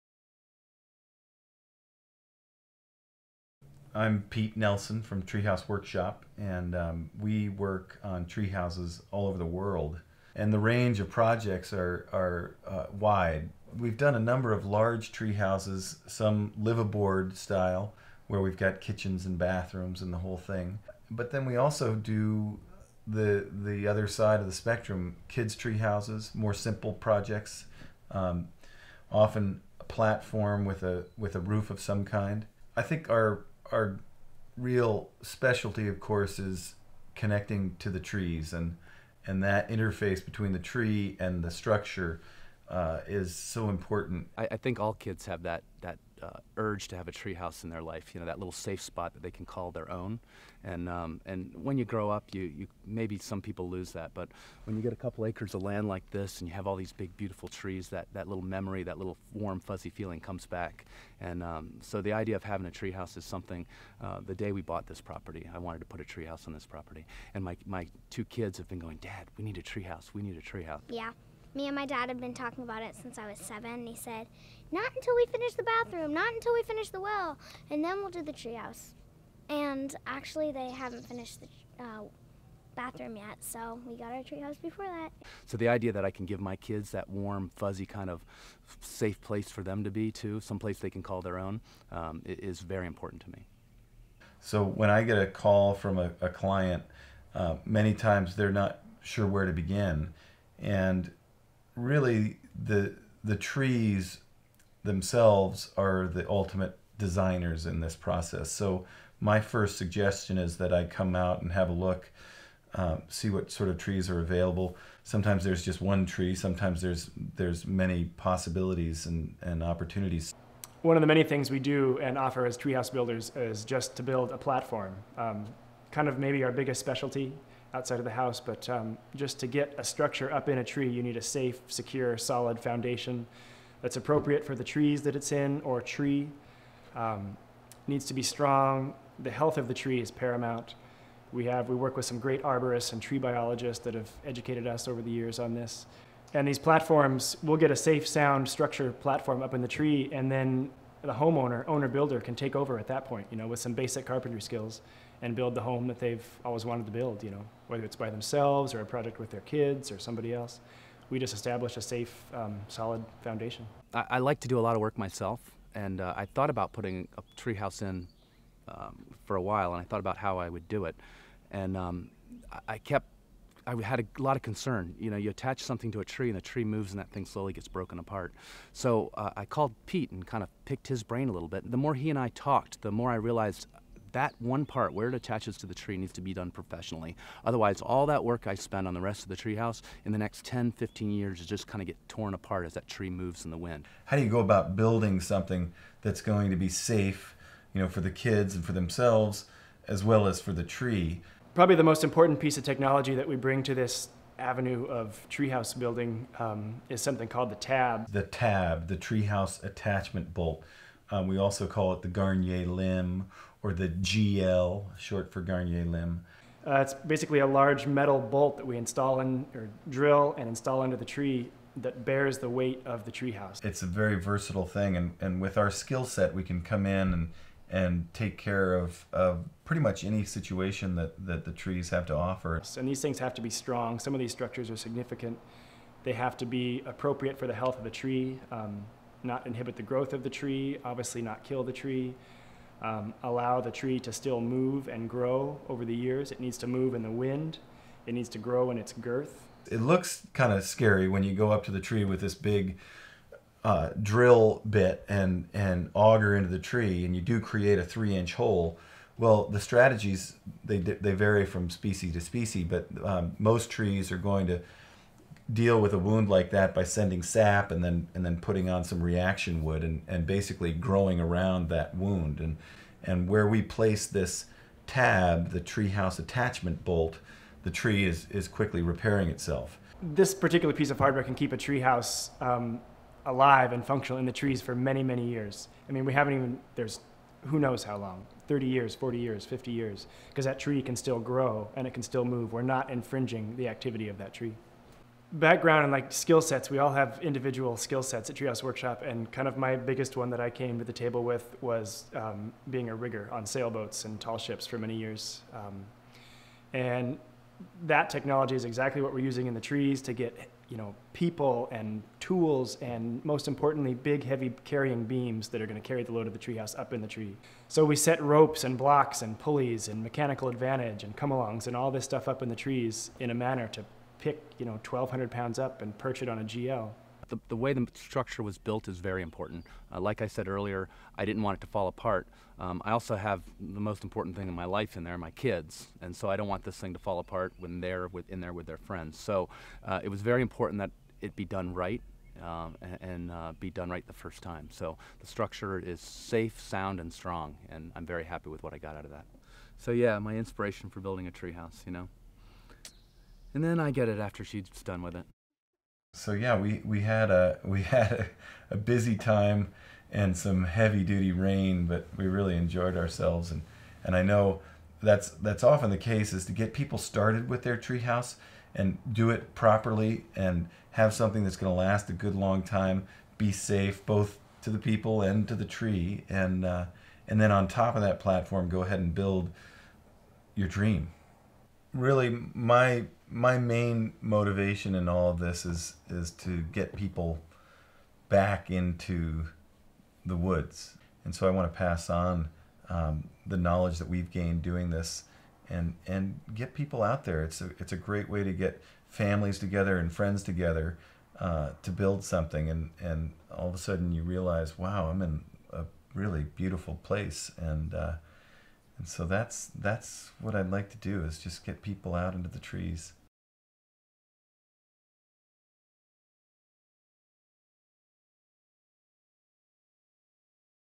Pete Nelson: Treehouse platform building
Pete Nelson and Ian Weedman discuss a platform project they worked on during 2011. Great footage of build progress and discussion of TABs (treehouse attachment bolts). For more information about treehouse plans, designing, consulting, building, and how-to workshops,